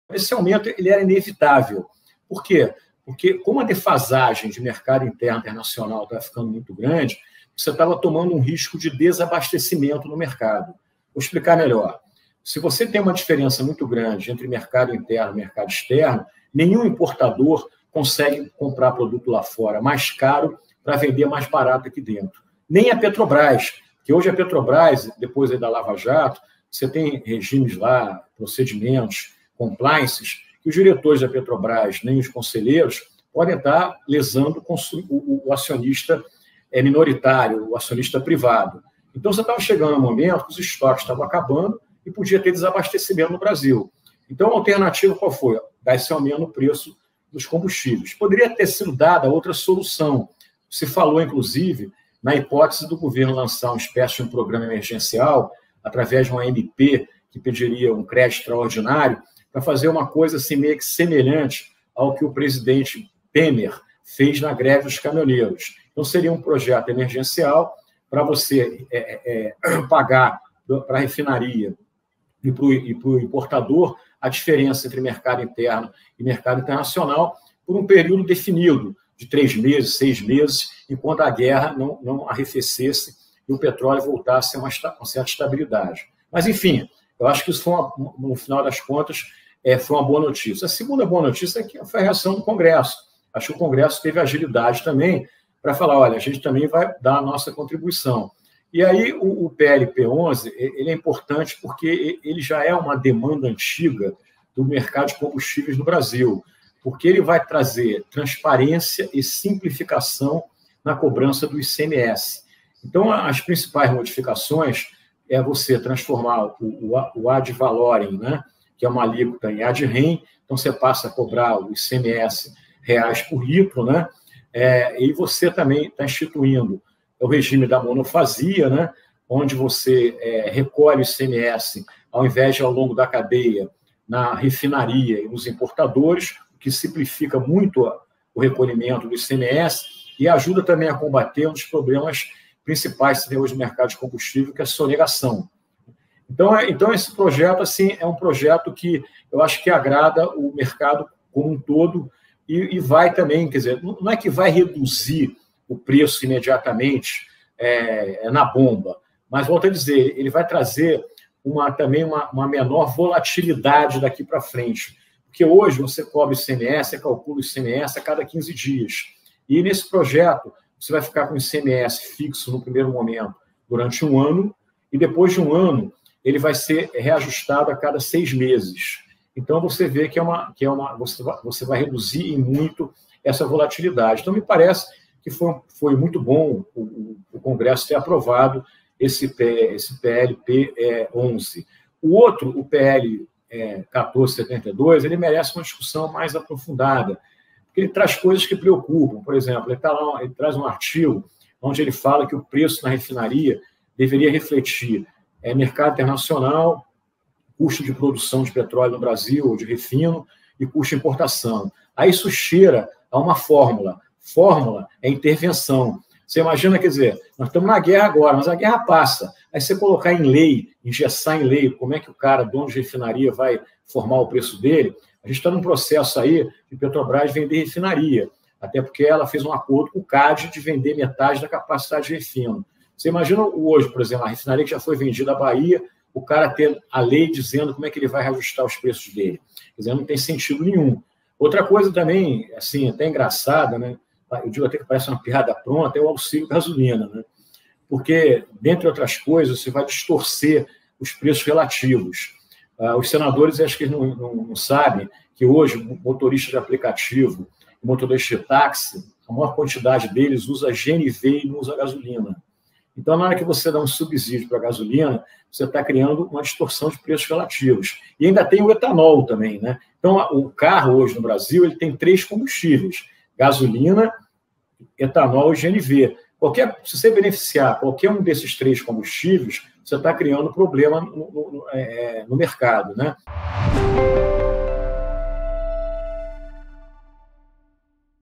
Adriano Pires fala sobre o aumento no preço dos combustíveis
00:00 - Por que o aumento dos preços era inevitável.
00:48 - Outras alternativas ao aumento dos combustíveis.
01:31 - A contribuição do congresso por meio da PLP11/2020.
02:26 - Como o PLP11/2020 ajuda a diminuir a controlar a volatilidade dos preços.
03:12 - PL 1472/21 e as discussões sobre precificação dos combustíveis.
04:01 - Considerações sobre o Auxílio Gasolina.